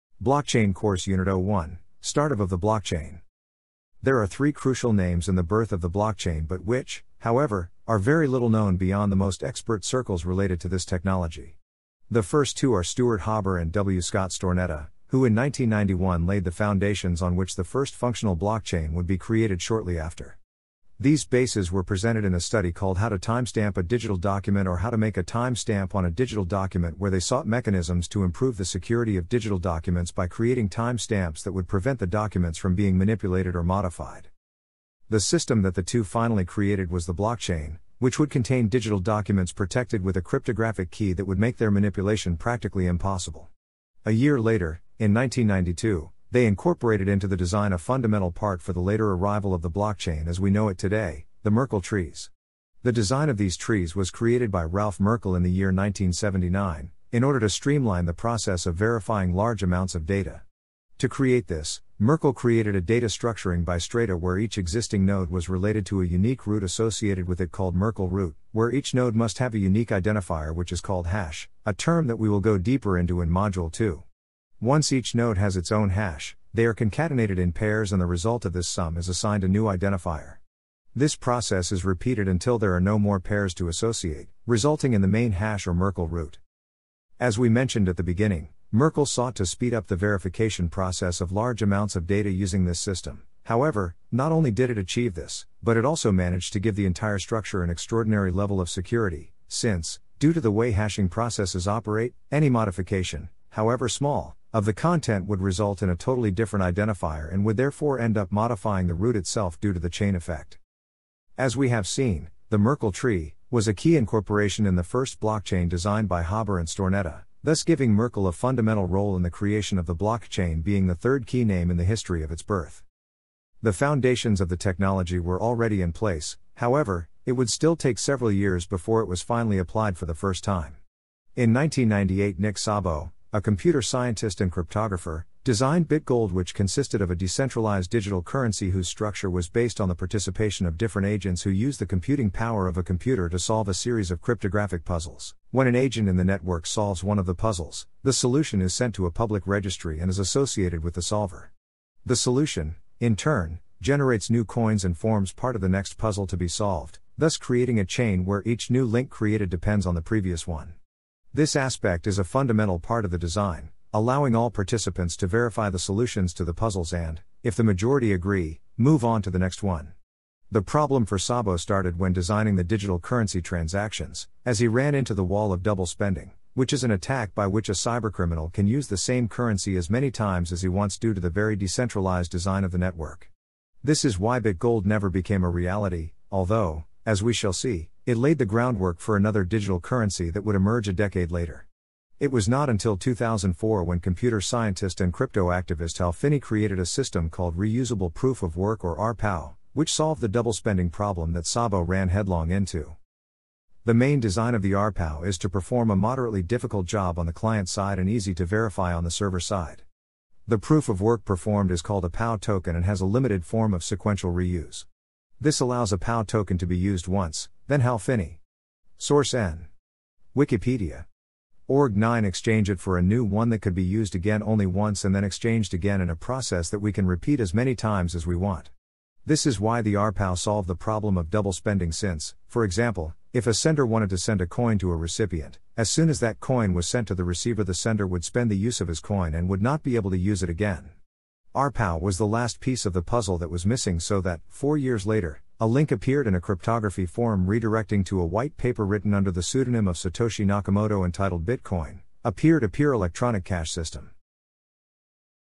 BLOCKCHAIN COURSE UNIT #01 START OF THE BLOCKCHAIN
✅ The blockchain is a distributed database that allows for secure, transparent and tamper-proof record-keeping. This breakthrough technology is already having a major impact on a wide range of industries, from banking and finance to healthcare and supply chain management.

The blockchain is often described as a “trustless” system, due to its ability to remove the need for third-party intermediaries. This is because the data on the blockchain is immutable and can be verified by anyone with an internet connection.

This makes it ideal for applications where transparency and security are paramount, such as in the case of financial transactions. The blockchain also has the potential to streamline supply chain management and make other business processes more efficient.

One of the most exciting aspects of the blockchain is its ability to enable “smart contracts”. These are contracts that can be automatically executed when certain conditions are met. This could have a huge impact on a wide range of industries, from insurance to real estate.

The blockchain is still in its early stages, but its potential is already clear. This transformative technology is poised to change the way we do business and interact with the world around us.